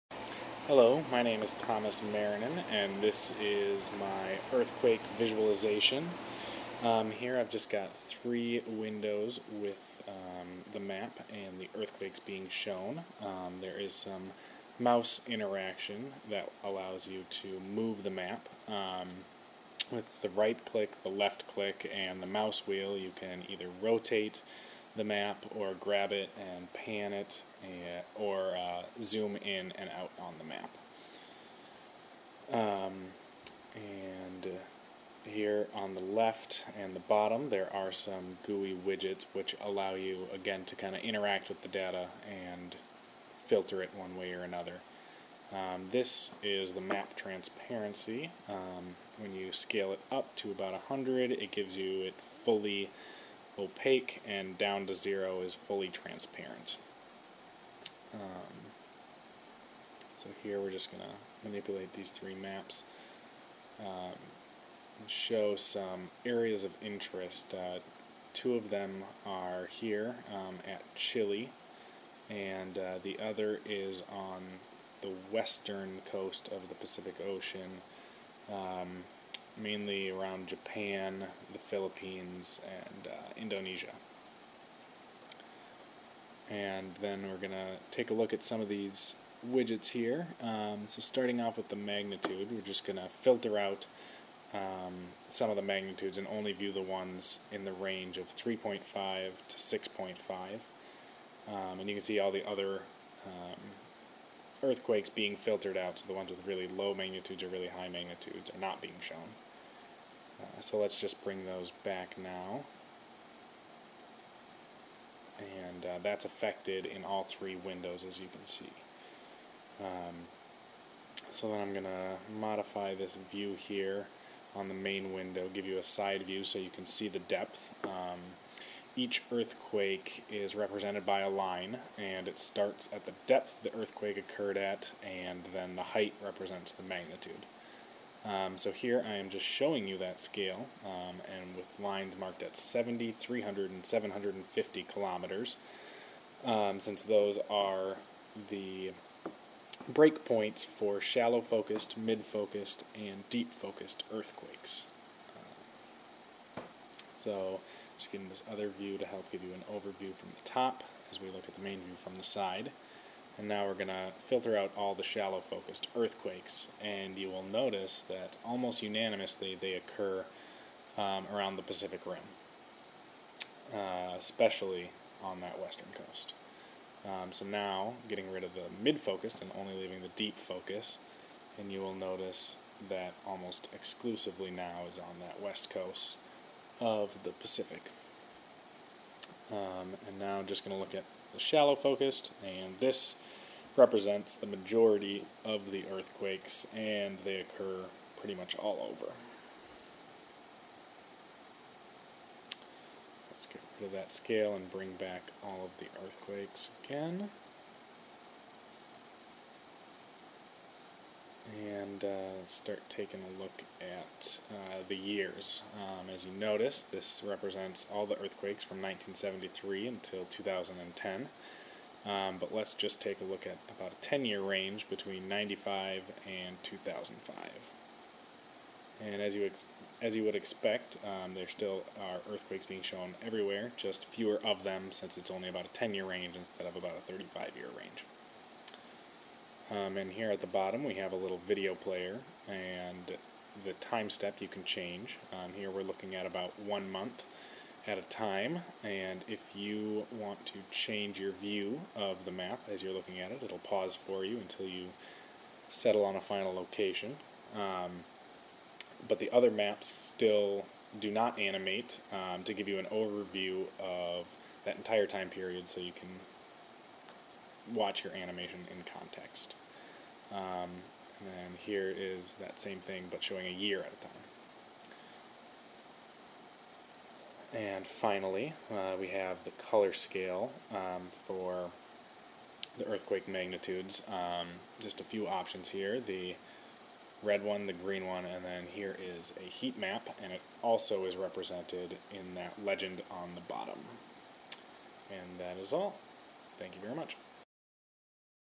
VTK Earthquake Demo
A 3d visualization of earthquakes occuring from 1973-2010.  This application was built using VTK (the Visualization ToolKit) and utilizes a Qt GUI interface.  It allows users to explore earthquakes based on the location of their hypocenter, their magnitude, and the time when they happened.